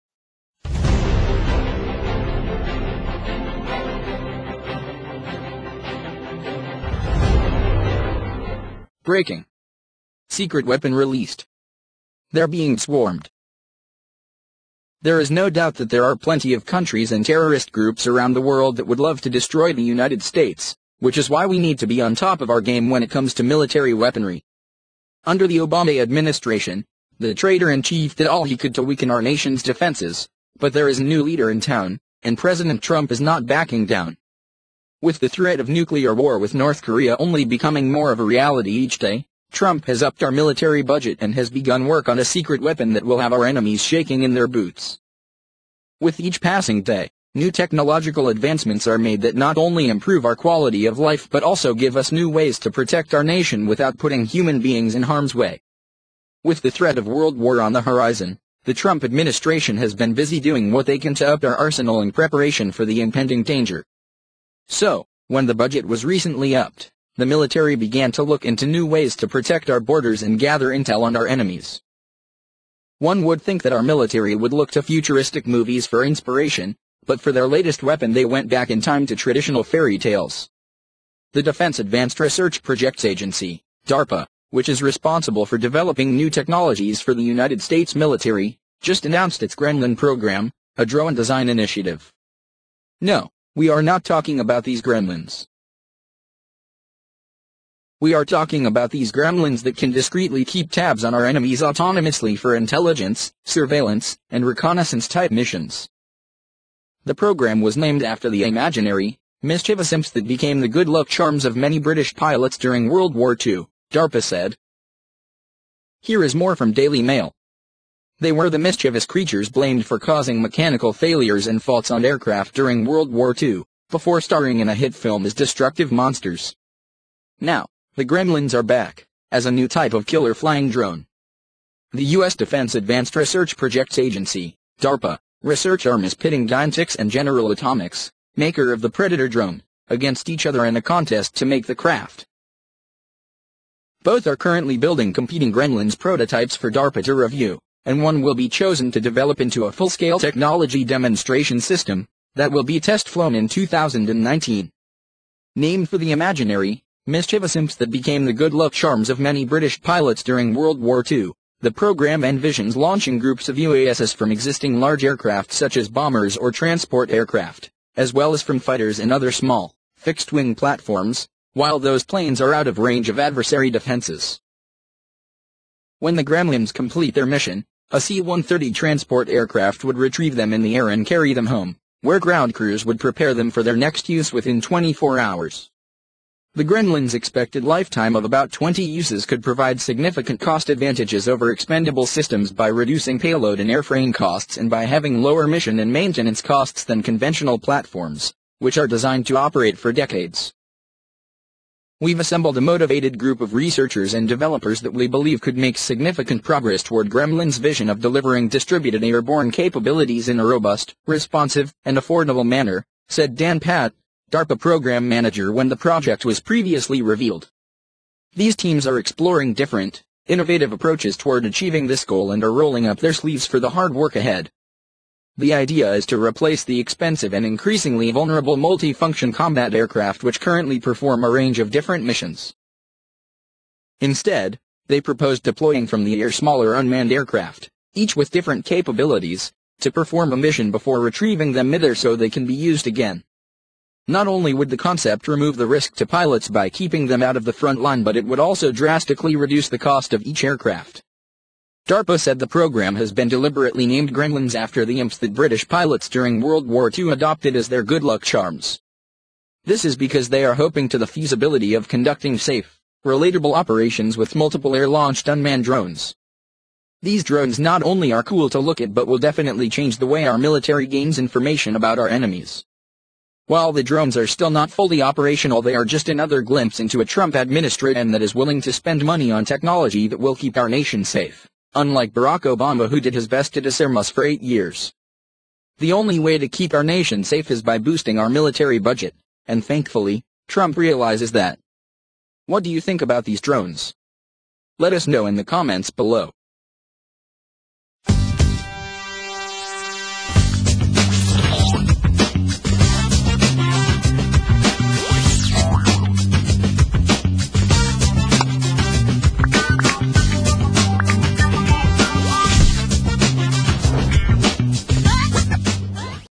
BREAKING! United States Military Secret Weapon RELEASED! They’re Being SWARMED in 2018 in the world
USA BREAKING NEWS TODAY
One would think that our military would look to futuristic movies for inspiration, but for their latest weapon they went back in time to traditional fairy tales.

The Defense Advanced Research Projects Agency (DARPA), which is responsible for developing new technologies for the United States military, just announced its “gremlin” program, a drone design initiative.
Wounderfull pak Show a world of wounderfull best videos